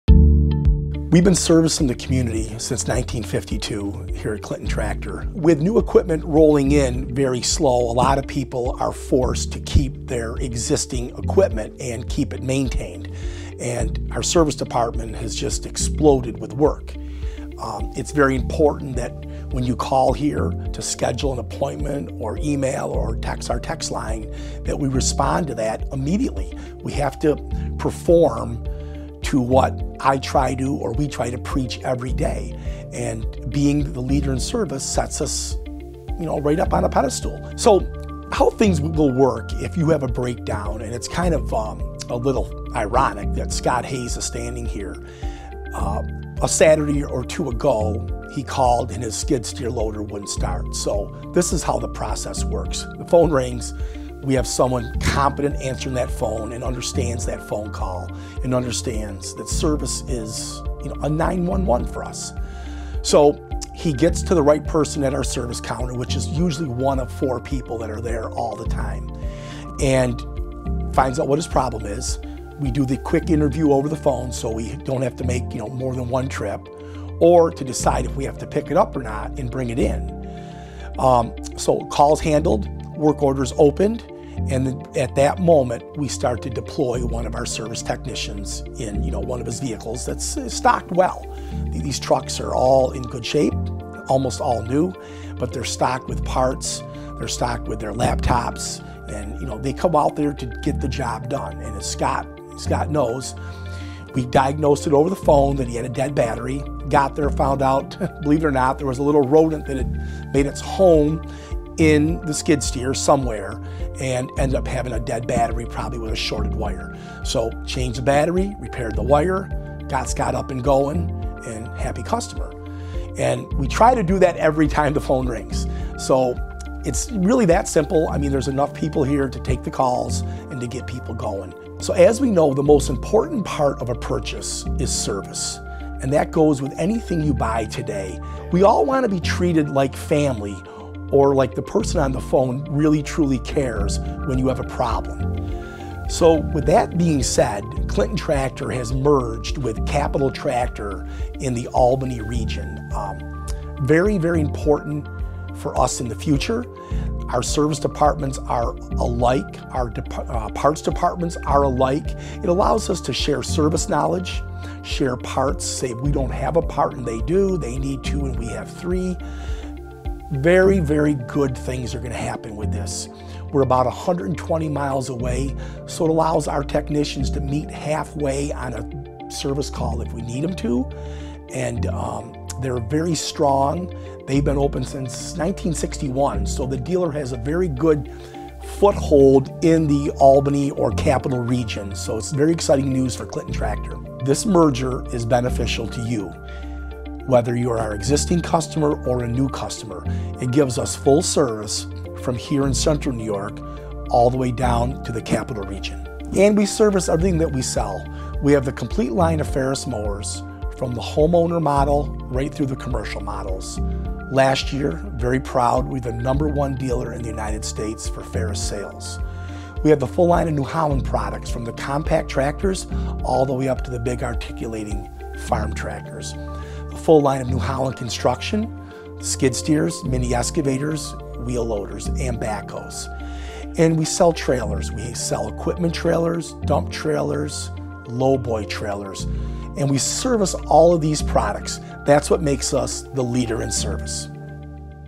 Clinton Tractor & Implement Co. 2022
A visit to Clinton Tractor and Implement Co. to learn about what they offer and what makes them "the leader in service!"